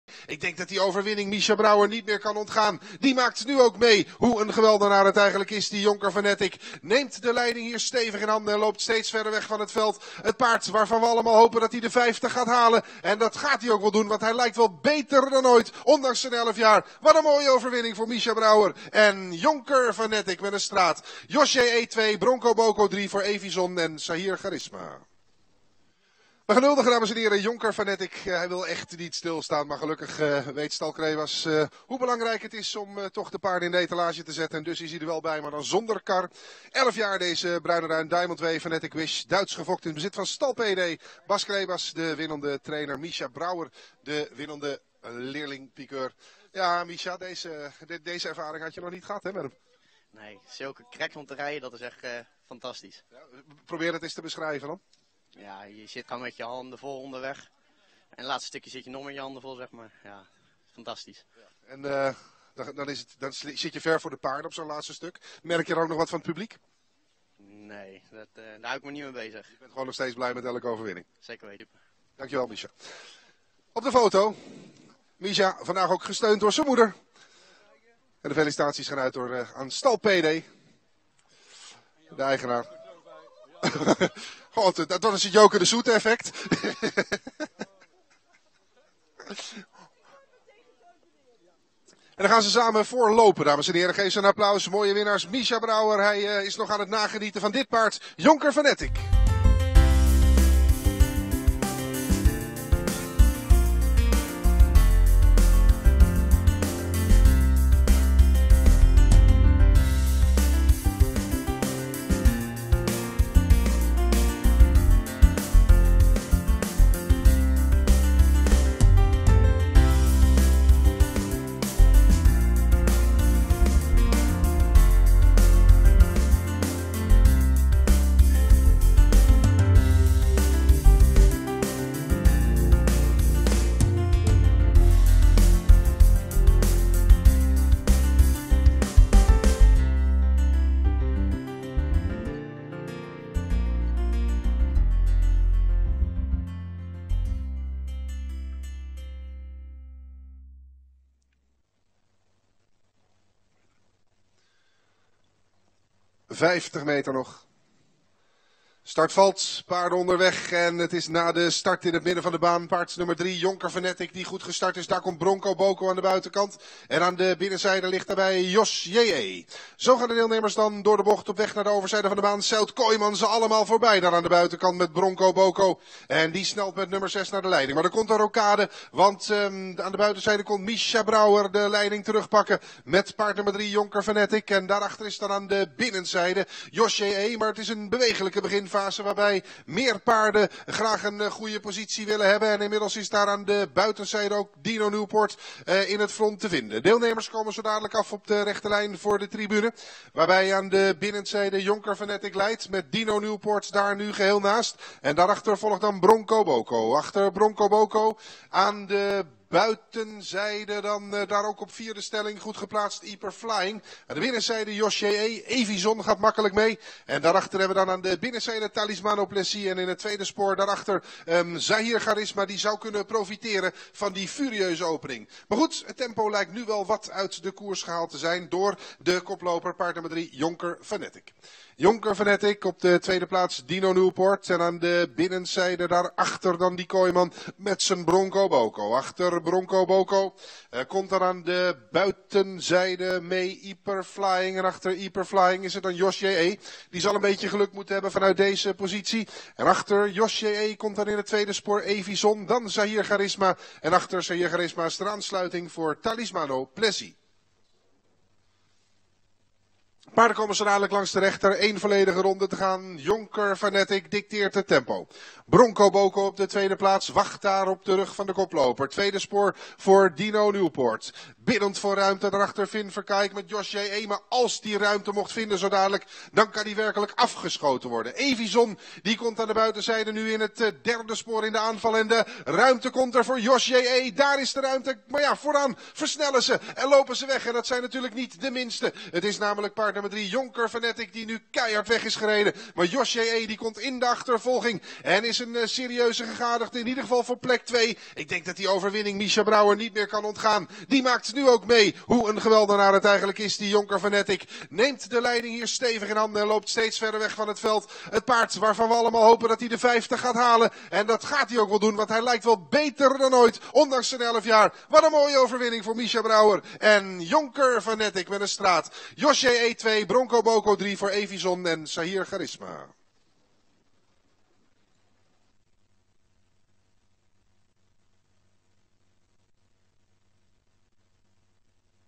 Yonker Fanatic & Micha Brouwer winnen op Drafcentrum Alkmaar - 17 okt. 2016
Yonker Fanatic & Micha Brouwer winnen de "Upper Frejo-prijs" voor Payday Racing en tr. Bas Crebas op Drafcentrum Alkmaar - 17 okt. 2016
Eig. Yonker Fanatic: Stal Payday - Peter Delis